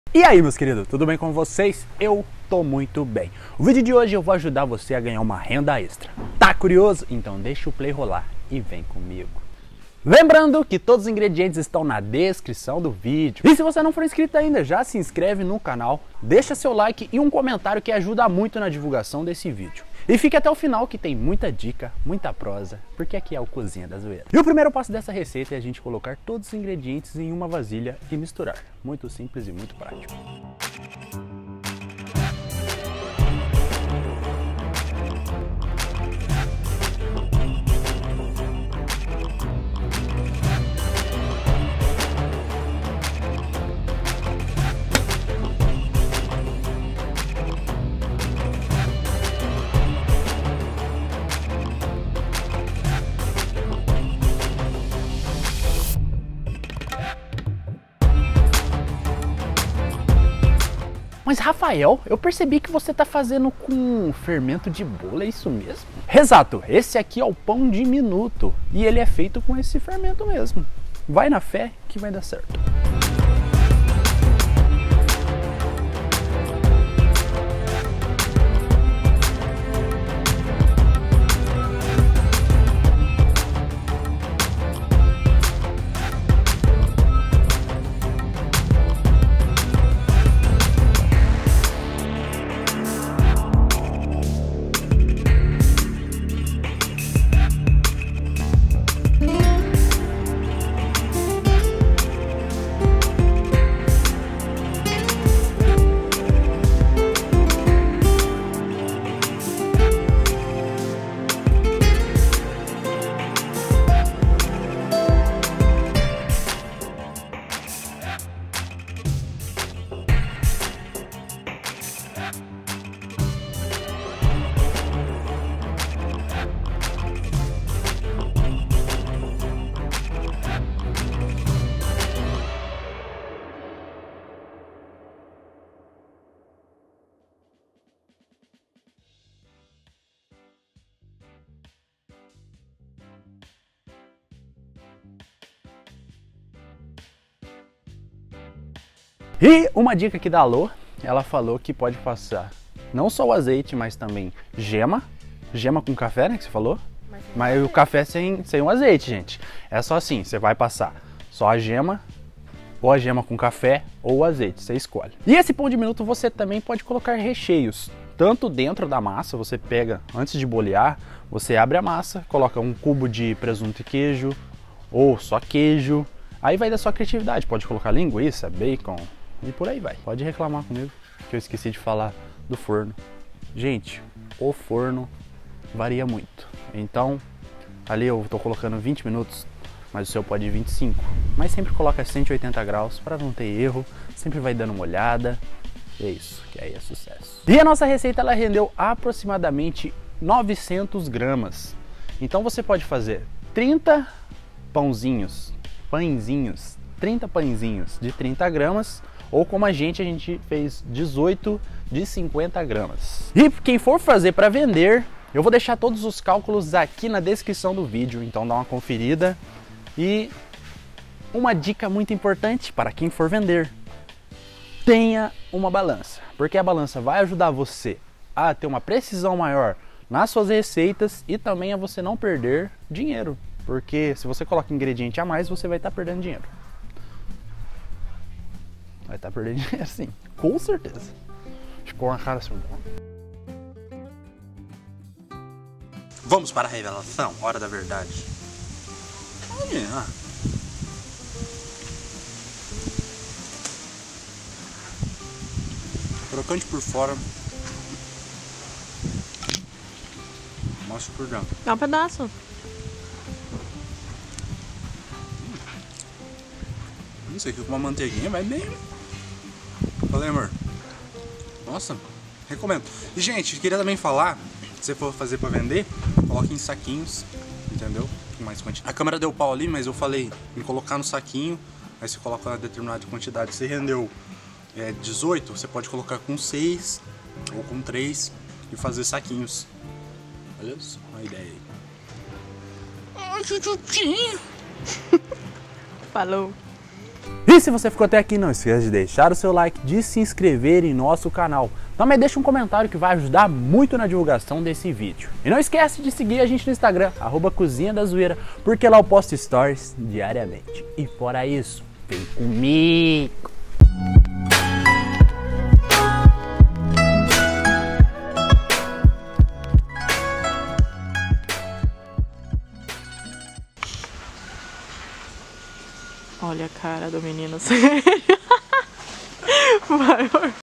FATURE PELO MENOS 15 REAIS POR FORNADA DESSE PÃOZINHO| PÃO DE MINUTO
Ingredientes:
500g de farinha de trigo ( 3 xícaras)
30g de açúcar (2 colheres de sopa)
5g de sal (1 colher de café)
20g de fermento químico (1 colher de sopa)
240 ml de leite (1 xícara)
60g de margarina (2 colheres bem cheias)
1 ovo
Nível da receita: Fácil
Cálculo de custo= 1,42 (farinha)+ 0,06(açúcar)+0,01 (sal)+0,59(fermento químico)+0,57(leite)+0,71(margarina)+0,46(ovo)= 3,82

0,21 centavos custo de insumos por unidade de 50g.
Sempre gosto de multiplicar por 3,5 ou 4 o valor do custo (1 parte é o gasto, 1 parte é a mão de obra, 1 parte é o custo fixo+impostos e a ultima parte o lucro) dá para vender 3 pães por 2,50 tranquilamente, dependendo da embalagem você pode agregar até mais valor, e melhor ainda se colocar um recheio.
Este vídeo é para você que quer uma ideia do que pode fazer para faturar uma renda extra, gastando bem pouco. Então nesse vídeo ensino como fazer um pão de minuto, muito simples e saboroso. Aprenda e fature pelo menos 15 reais por fornada que fizer desse pão de minuto!